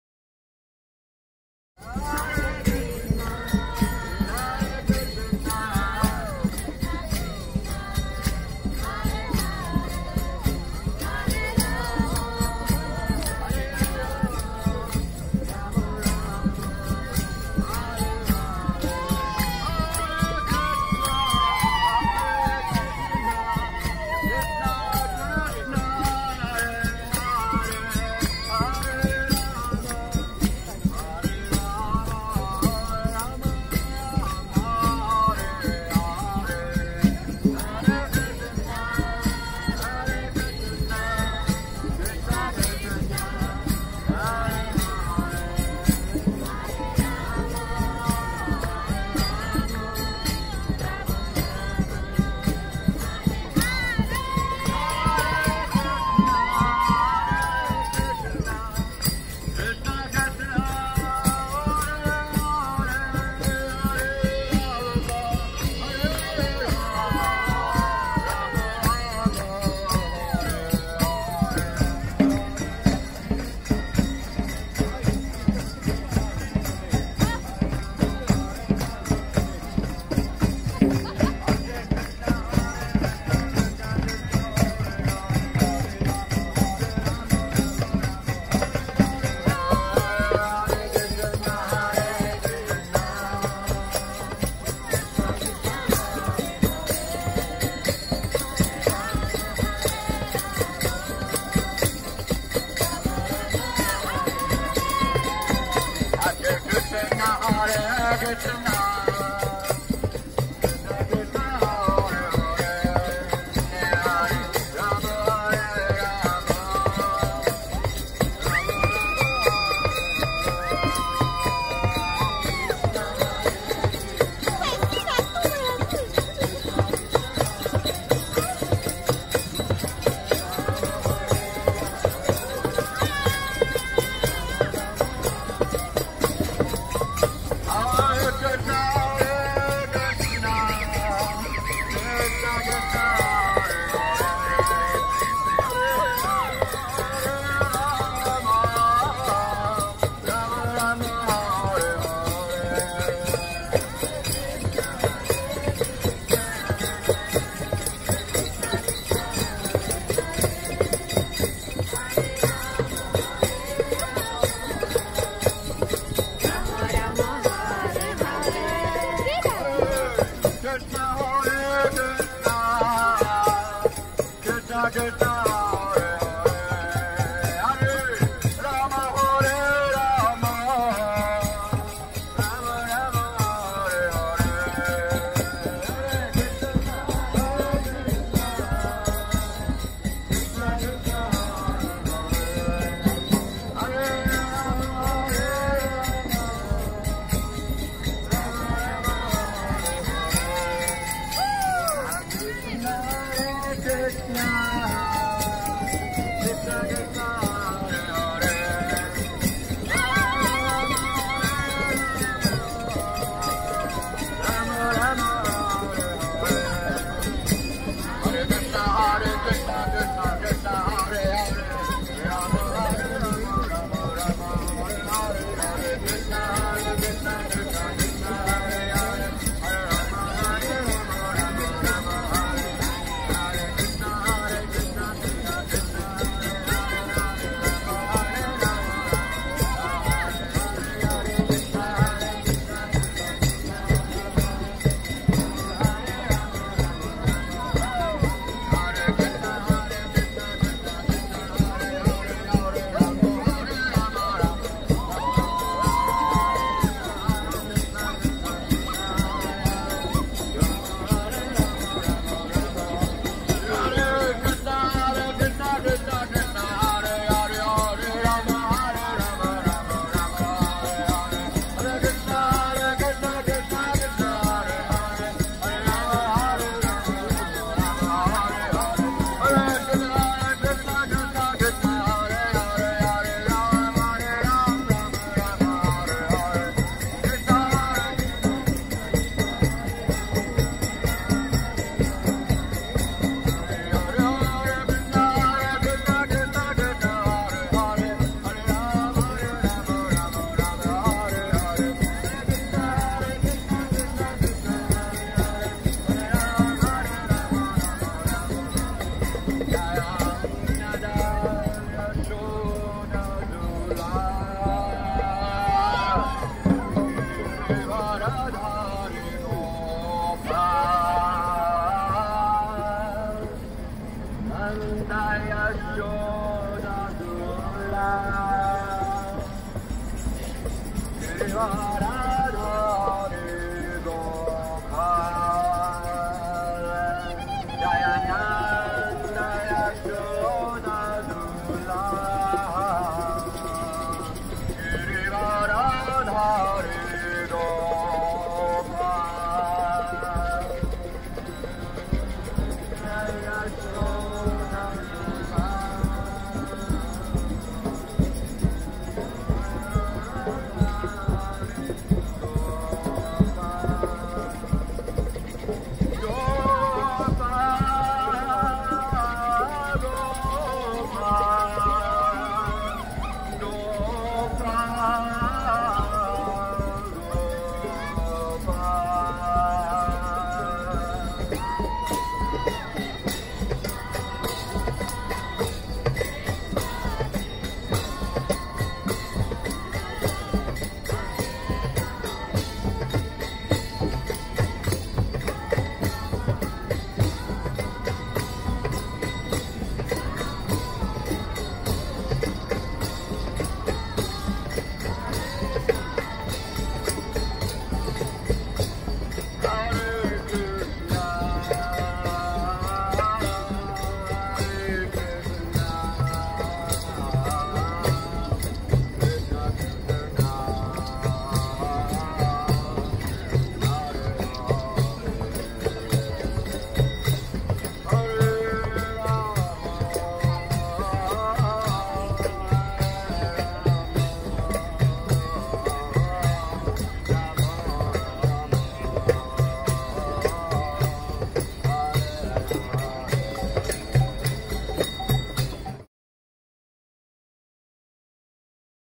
Fire Performance at Helsinki Railway Station Square 2011 filmed with Nokia N9
Did not have my "proper" video camera with me but no problem as modern mobile phone can do the job quite well considering the "low light condition".  Resolution downscaled a bit from original 720p.